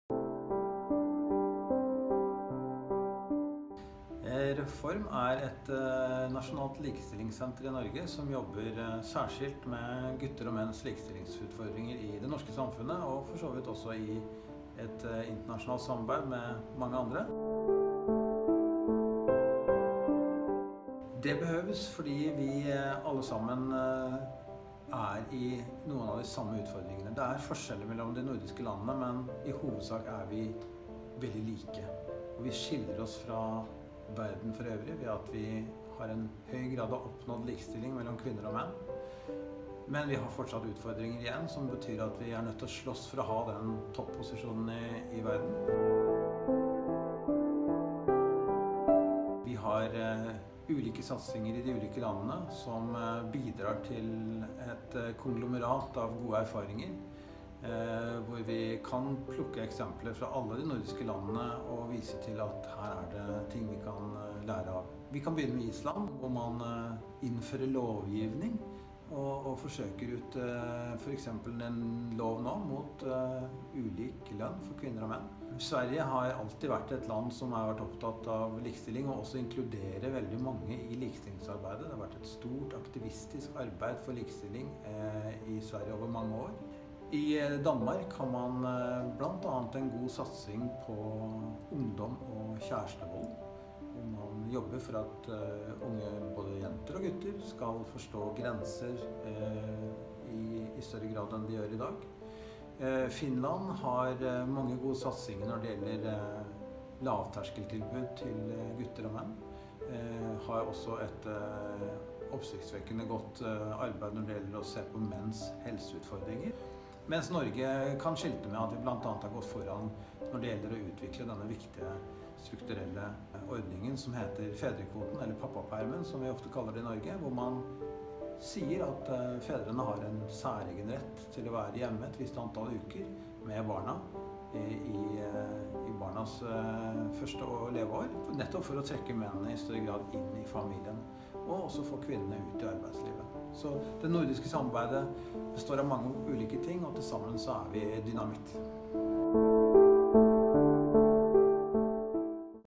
NIKK talks to Reform about gender equality in the Nordics
Are Saastad is the head of Reform, a Norwegian resource center for men. The Nordic countries have come a long way in achieving gender equality. In this film, Saastad is giving examples of some of the efforts made in the different countries.